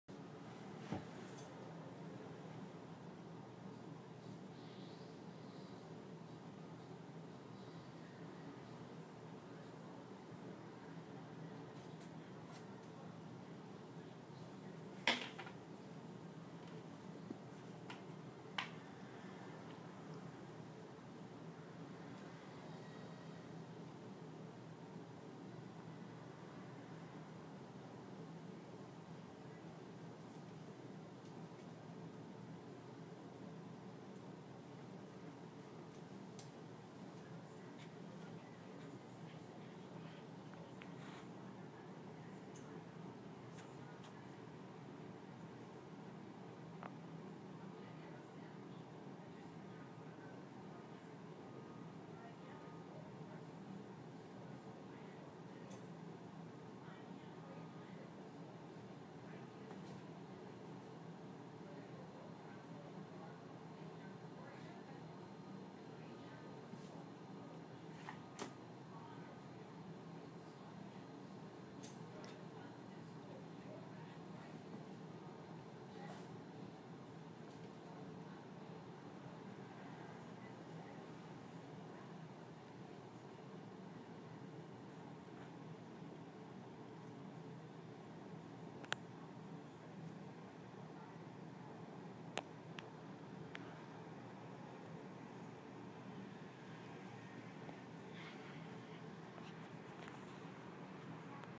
Wil Learns How to Open Drawers
We hide some of Wil's toys because they turn him into a scary, full-blown hunter. He found out where we hid them and decided to take matters into his own paws.